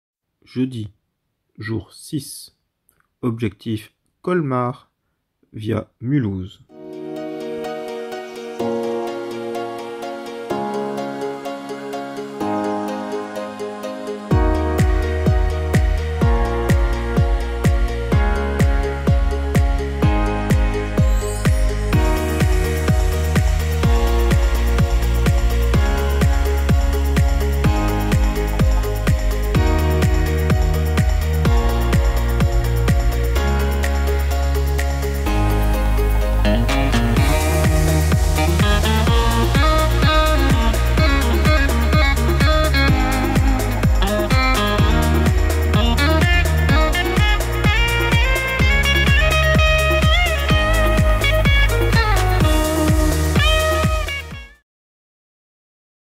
[MINIVIDEO #6] Gyrotrip 2021 / saison 4 - jour 6
Gyrotrip 2021
Minividéo du jour 6

Passage par Mulhouse, Colmar

Chaque jour la mini-vidéo de la veille sur notre chaîne YouTube avant le montage des vidéos complètes.